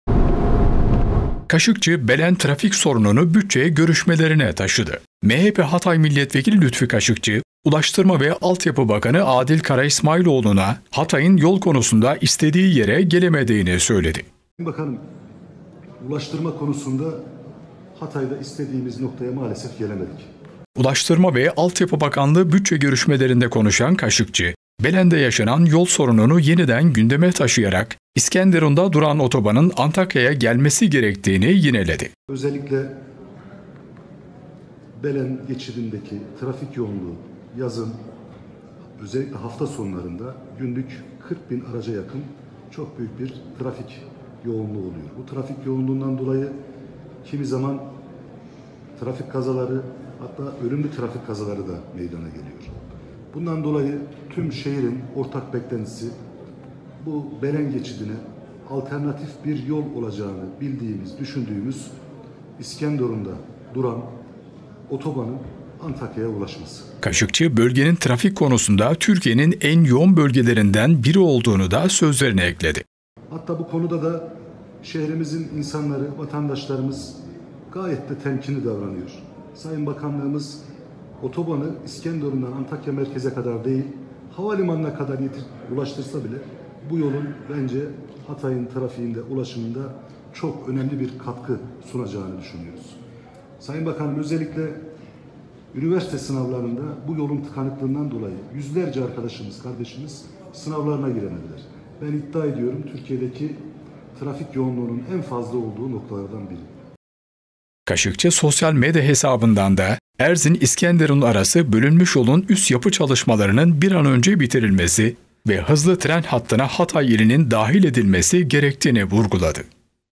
KAŞIKÇI, BELEN TRAFİK SORUNUNU BÜTÇE GÖRÜŞMELERİNE TAŞIDI
MHP Hatay Milletvekili Lütfi Kaşıkçı Ulaştırma ve Altyapı Bakanı Adil Karaismailoğlu’na Hatay’ın yol konusunda istediği yere gelemediğini söyledi.

Ulaştırma ve Altyapı Bakanlığı bütçe görüşmelerinde konuşan Kaşıkçı Belen’de yaşanan yol sorununu yeniden gündeme taşıyarak, İskenderun’da duran otobanın Antakya’ya gelmesi gerektiğini yineledi.

Sakın Kaçırmayın
BEYZADE ANA HABER BÜLTENİ
Her akşam Beyzade Ana Haber bülteni ile karşınızdayız...